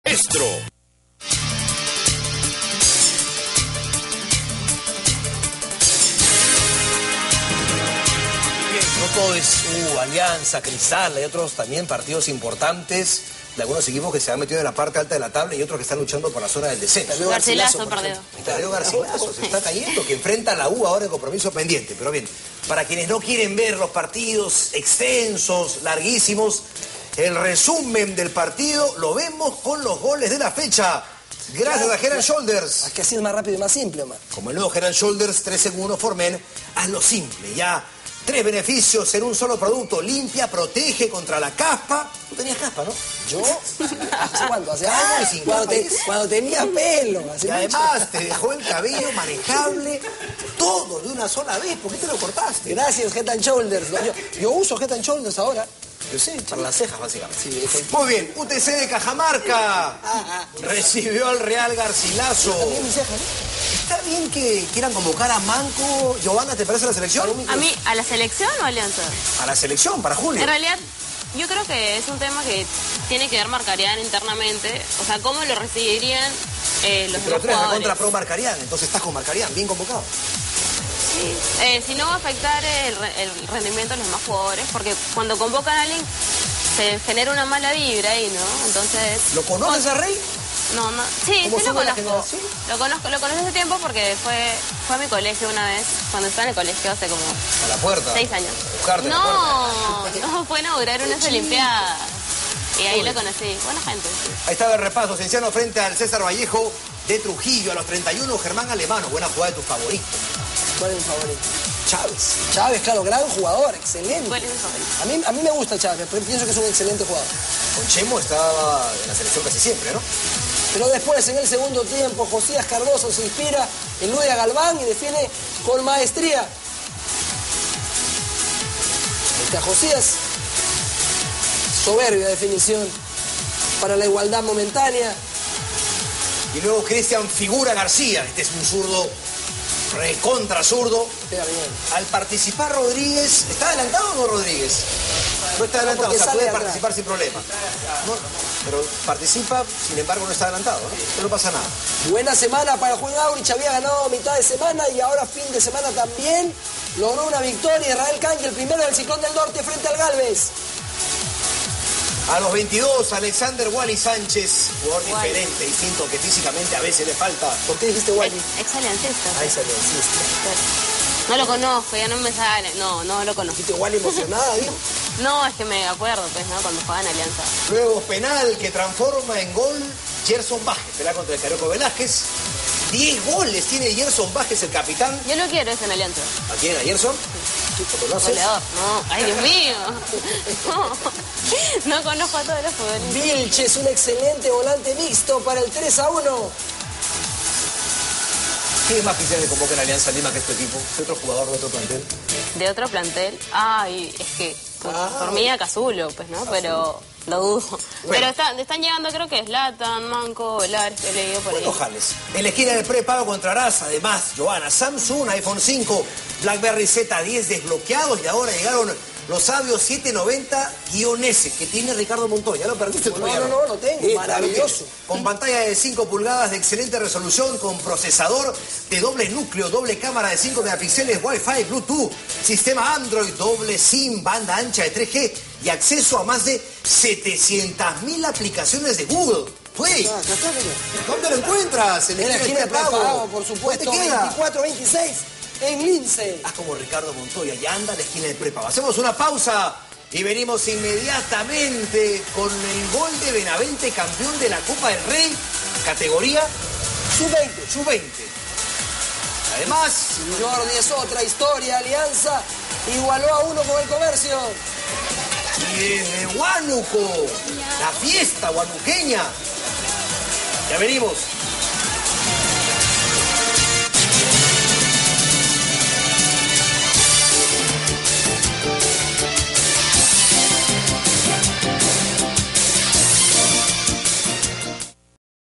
Torneo Descentralizado 2013: resumen de los encuentros de la fecha 22
UTC 1-0 Real Garcilaso. Pacífico 0-0 Melgar. Cienciano 2-1 César Vallejo. Juan Aurich 3-1 José Gálvez.